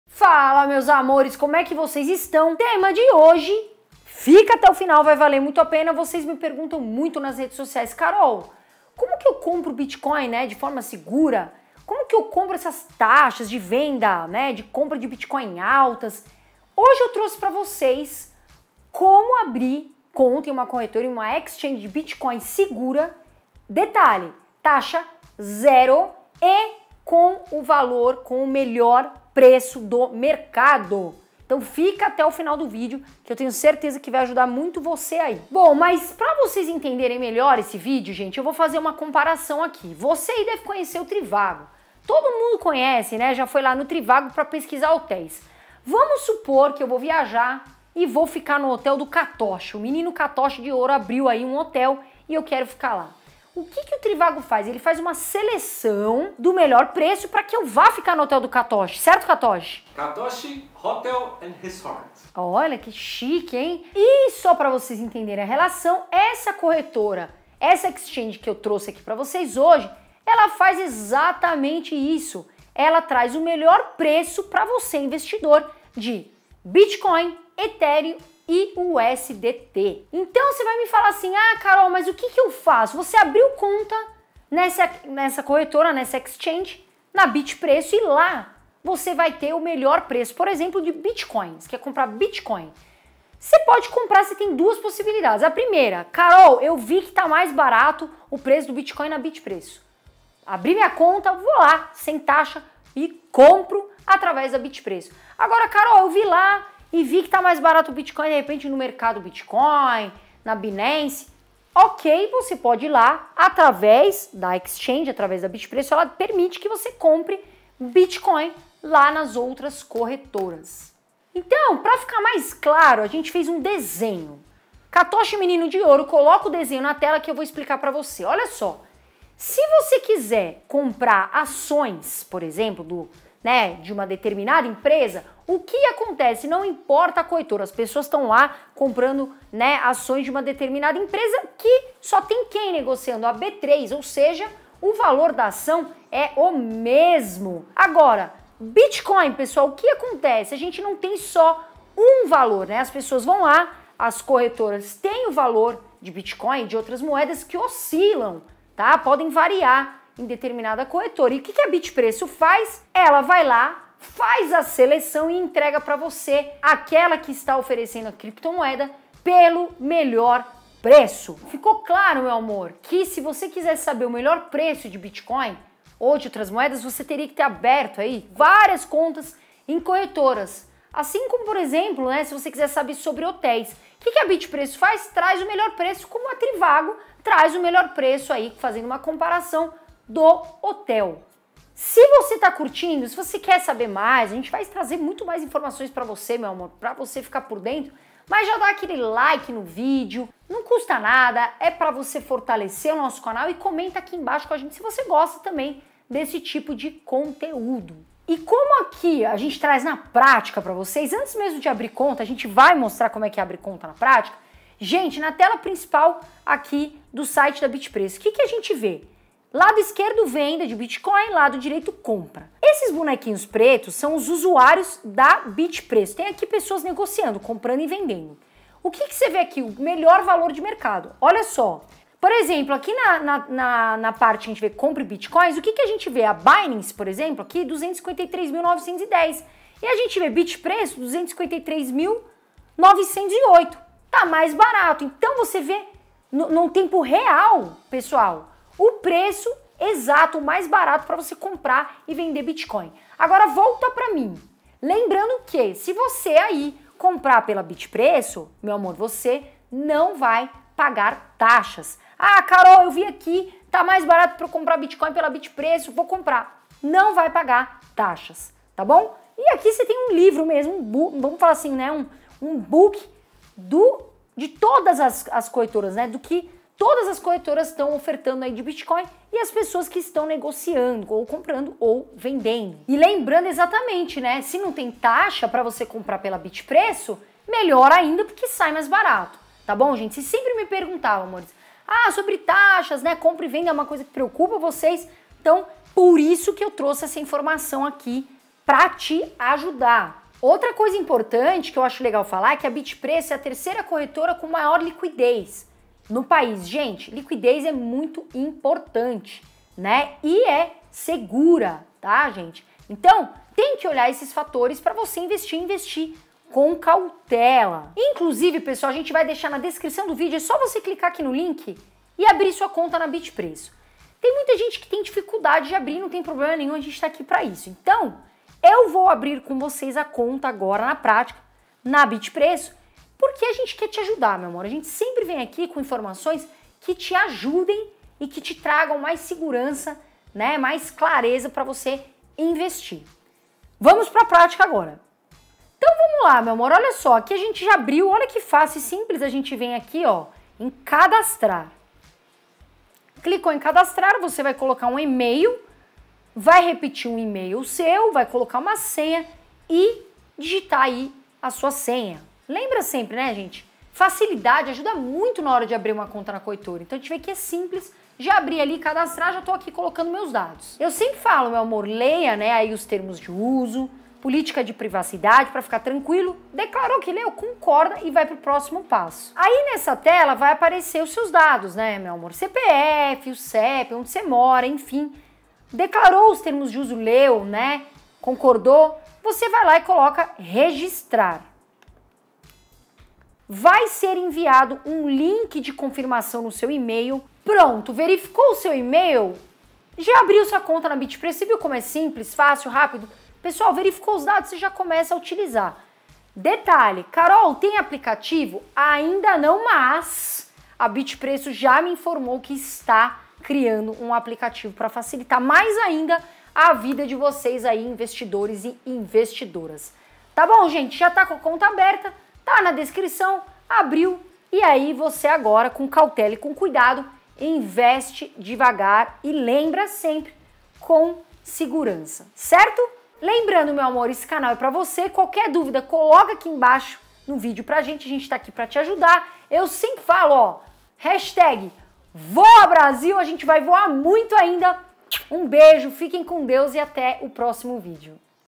COMO COMPRAR BITCOIN? | Onde INVESTIR em BITCOIN com SEGURANÇA?
📲 ABRA SUA CONTA GRÁTIS NA BITPREÇO

Onde investir em Bitcoin com segurança? Você vai aprender como investir em Bitcoin pelo menor preço e com taxa zero na exchange (corretora) BitPreço que eu estou utilizando agora.

A BitPreço é um Market place de criptomoedas e dentro dela você consegue ver o preço do Bitcoin, Etheriun, USDT em várias outras corretoras e comprar na que estiver sendo negociada mais barato. Na BitPreço você consegue ver em qual corretora o bitcoin está sendo negociado pelo menor preço e comprar na que ele está mais barato.